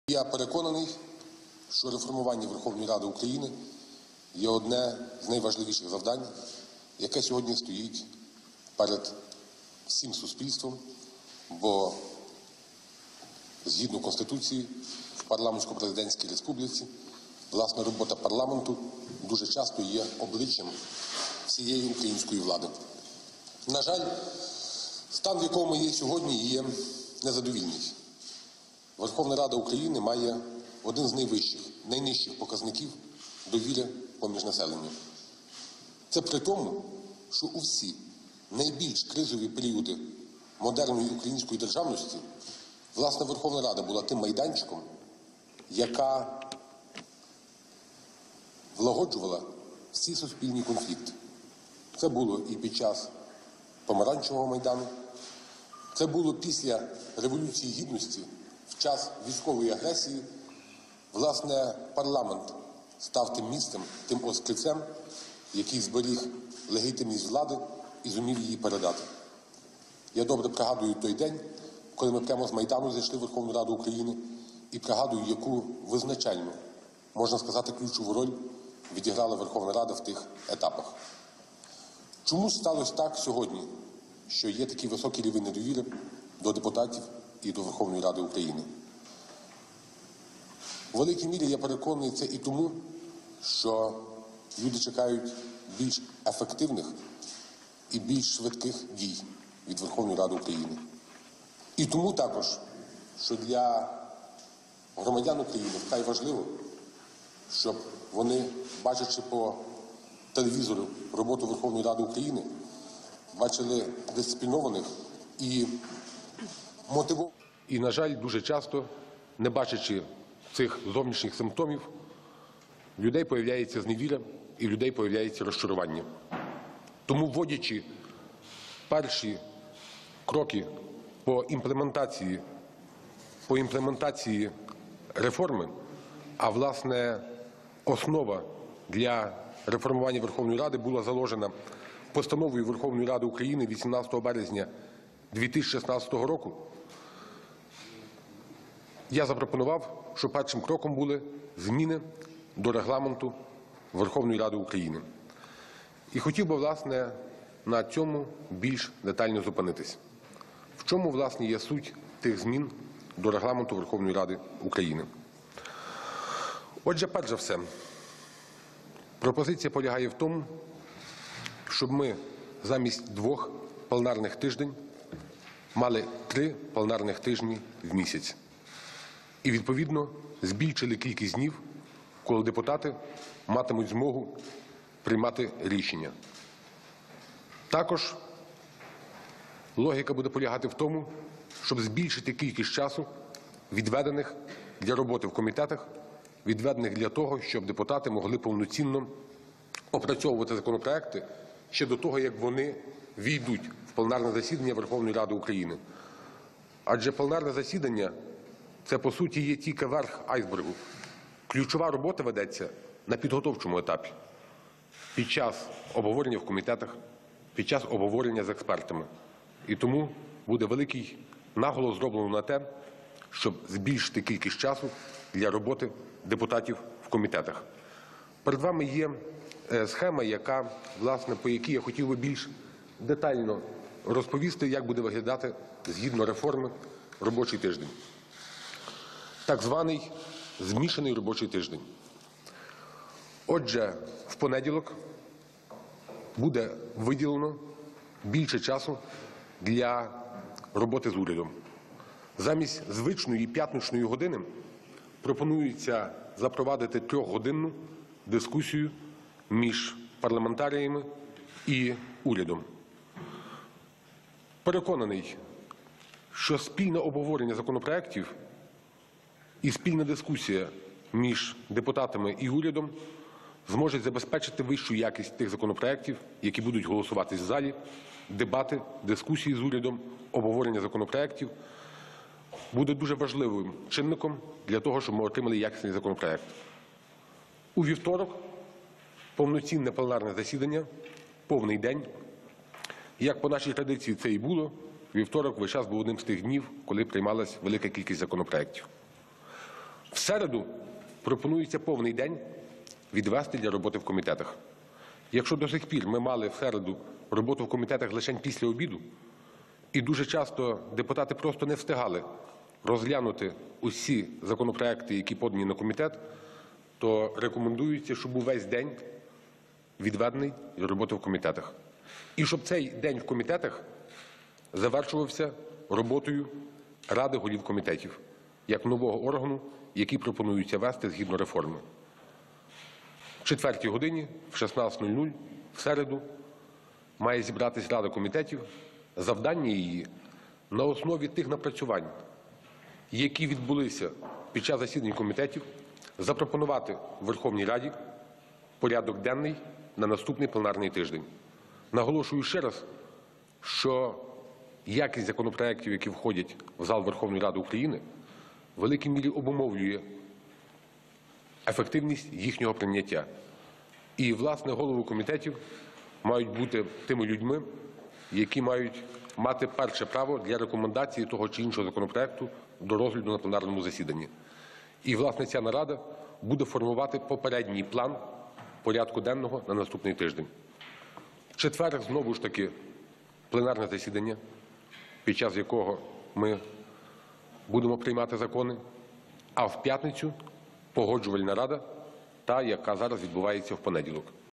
Андрій Парубій презентував реформу Верховної Ради
Голова Верховної Ради, представник «Народного фронту» Андрій Парубій презентував основні положення реформи парламенту як найвищого законодавчого органу.

«Я переконаний, що реформування Верховної Ради України є одним із найважливіших завдань, яке сьогодні стоїть перед усім суспільством, бо, згідно Конституції, у парламентсько-президентській республіці парламент є обличчям усієї української влади», - підкреслив Голова Парламенту, повідомляє прес-служба ВР.

Зазначаючи, що Верховна Рада «має сьогодні один з найнижчих показників довіри суспільства», Андрій Парубій звернув увагу на те, що «у найскладніші періоди модерної історії українського суспільства вона була тим майданчиком, на якому влагоджувались суспільні конфлікти і після Помаранчевого Майдану, і після Революції Гідності».

«А під час військової агресії власне парламент став тим острівцем, який зберіг легітимність влади і зумів її передати», - наголосив він.

На його думку, такий рівень недовіри до влади, до Верховної Ради зумовлений тим, що люди чекають ефективніших і швидших дій від органів влади.

«І тому для громадян України важливо бачити, щоб робота, яка транслюється, і самі депутати були дисциплінованішими і такими, що працюють для України, - зазначив Парубій. - Тому роблячи перші кроки з імплементації реформи Верховної Ради, я запропонував, аби першим - були зміни до Регламенту».

Голова Верховної Ради зосередив увагу представників засобів масової інформації на основних напрямках змін до Регламенту Верховної Ради, які полягають у наступному:

- Замість двох пленарних тижнів на місяць пропонується запровадити три пленарних тижні і відповідно збільшити кількість днів, коли депутати матимуть змогу приймати рішення.

- Пропонується збільшити кількість часу для роботи у комітетах для того, щоб депутати могли повноцінно опрацьовувати законопроекти ще до того, як вони увійдуть у програму засідань Верховної Ради. Адже пленарне засідання - це є по суті вершина айсбергу, бо справжня робота ведеться на підготовчому етапі, в комітетах, з експертами.

- Великий наголос буде зроблено на те, аби збільшити кількість часу для роботи депутатів в комітетах.